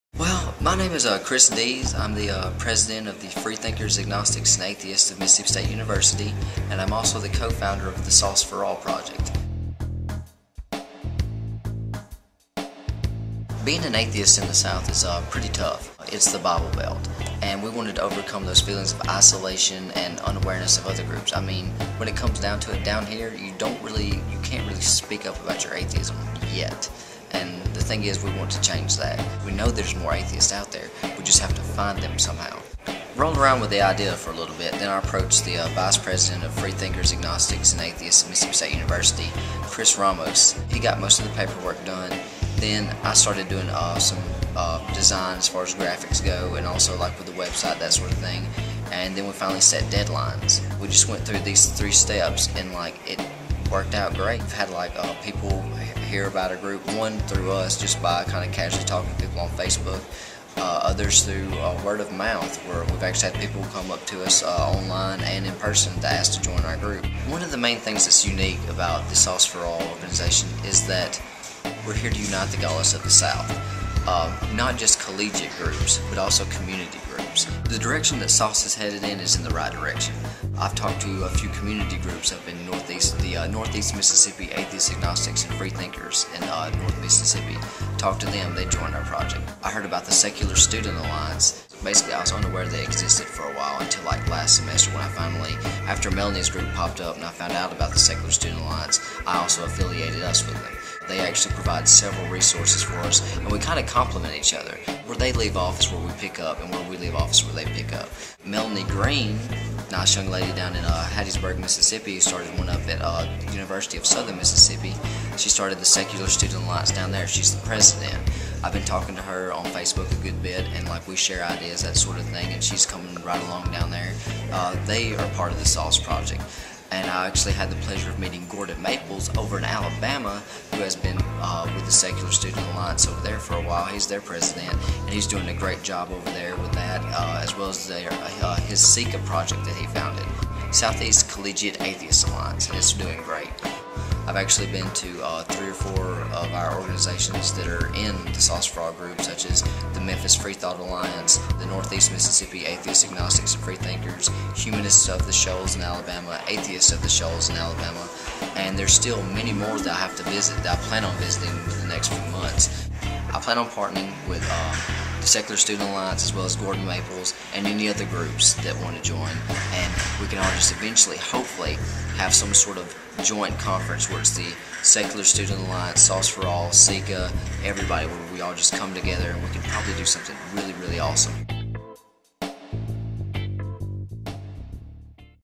What is Sauce For All ?
A introductory video to what the Sauce For All network is.

Sauce For All
Southern Atheists United for Candid Expression, Freethought, Openmindedness, Reason, And Living Life